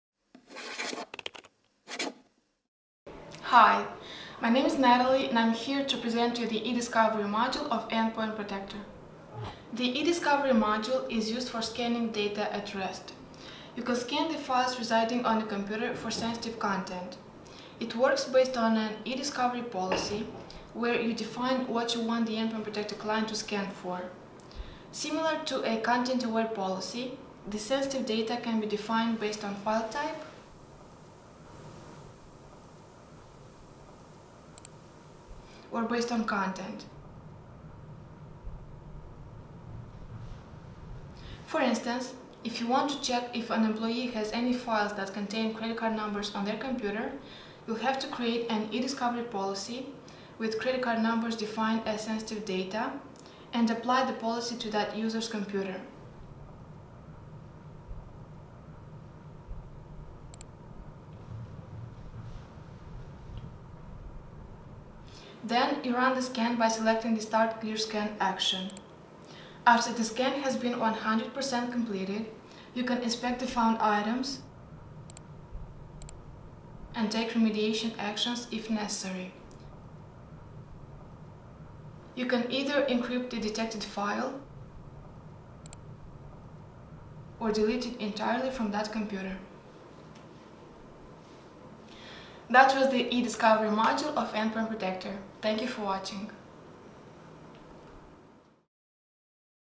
How eDiscovery Works?
eDiscovery scans and identifies the confidential information in organizations’ endpoints and allows Administrators to take remediation actions like encrypting or deleting data at rest. It addresses both internal and external threats – unauthorized employees storing sensitive data on their computers and attackers which manage to bypass the network defense and try to get a hold of the company’s records